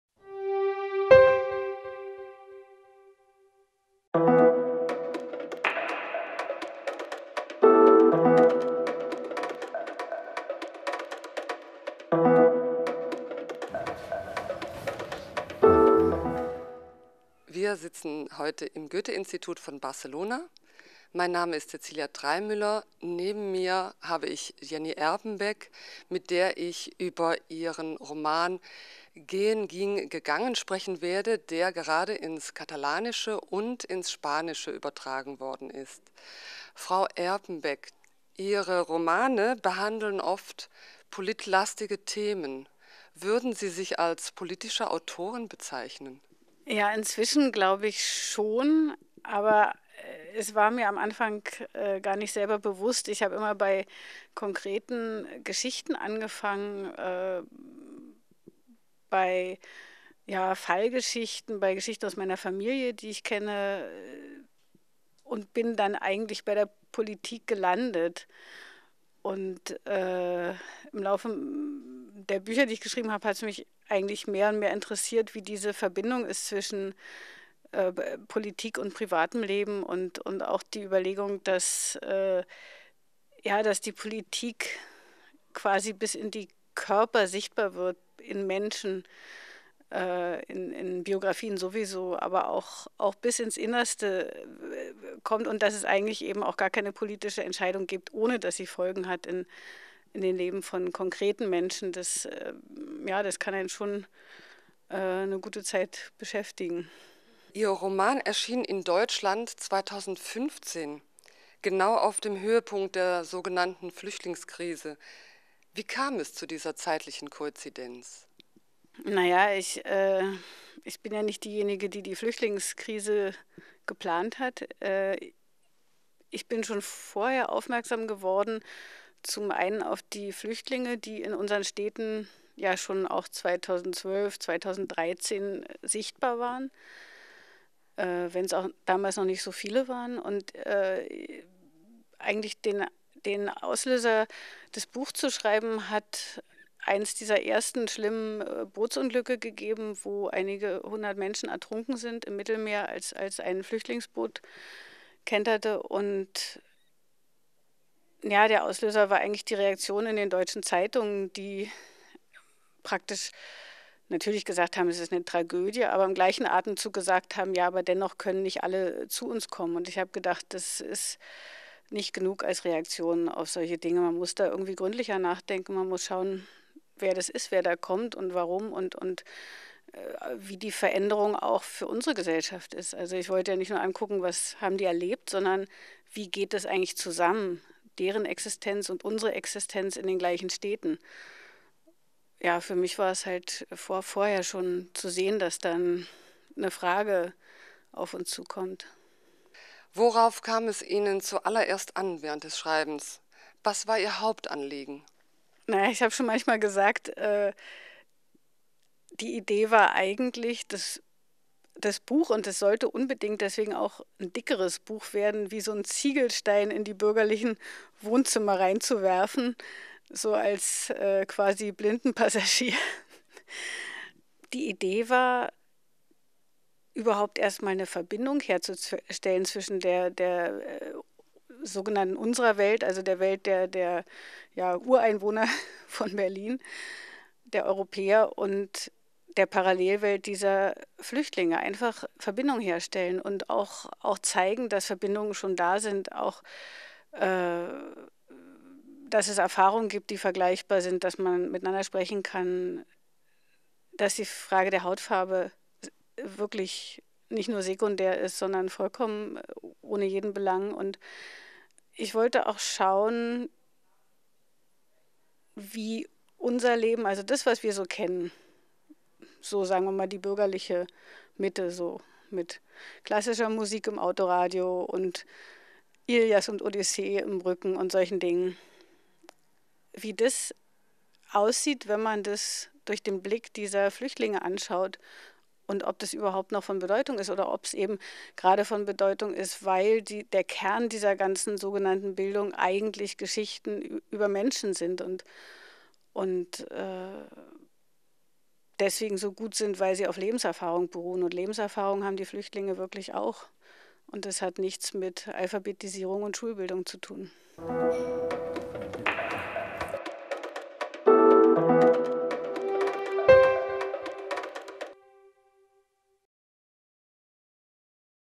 3 Fragen an Jenny Erpenbeck
Als die Schriftstellerin Jenny Erpenbeck ihren Roman Gehen, ging, gegangen 2015 in Deutschland publizierte, traf sie damit den Puls der Zeit: die Debatte über Flüchtlinge, die immer noch aktuell ist. Die Berlinerin veröffentlichte ein Buch, das von der Lebenssituation junger afrikanischer Männer in einem Flüchtlingscamp in der Hauptstadt Deutschlands erzählt.

3 PREGUNTAS A JENNY ERPENBECK
Jenny Erpenbeck publicó su novela Yo voy, tú vas, él va en 2015, tomando con ello el pulso de la actualidad del momento: el debate sobre los refugiados, que aún hoy sigue vigente. La novela de la berlinesa relataba la situación de vida de jóvenes africanos en un campamento de refugiados de la capital de Alemania.

3 PREGUNTES A JENNY ERPENBECK
Jenny Erpenbeck va publicar la seva novel·la Les formes del verb anar en 2015, prenent el pols de l'actualitat del moment: el debat sobre els refugiats, que encara segueix vigent. La novel·la de la berlinesa relatava la situació de vida de joves africans en un campament de refugiats de la capital d'Alemanya.